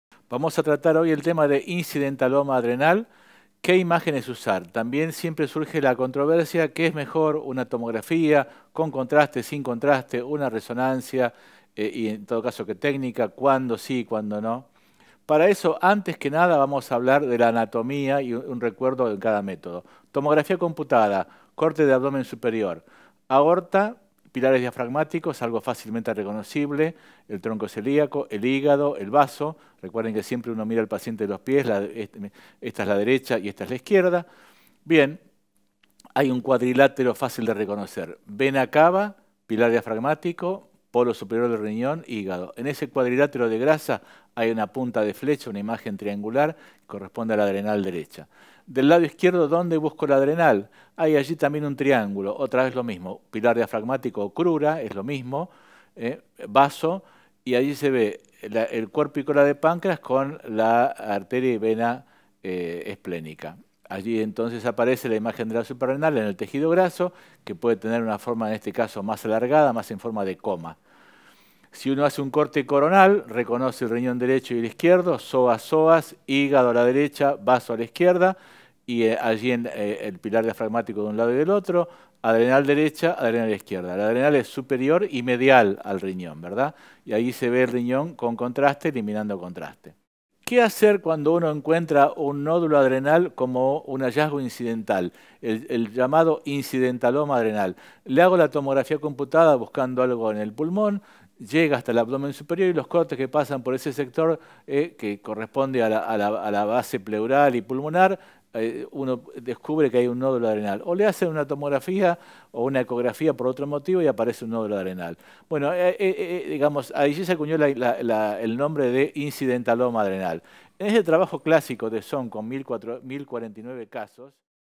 Incidentaloma Adrenal Que Imágenes Usar Dr José San Román Trailer
y accede a todo el contenido.

Dr. San Román, José - Profesor Titular de Diagnóstico por Imágenes de la Facultad de Medicina de la UBA (Universidad de Buenos Aires), Argentina. Médico Especialista en Diagnóstico por Imágenes. Médico Consultor de TCBA y Fundación J. Roca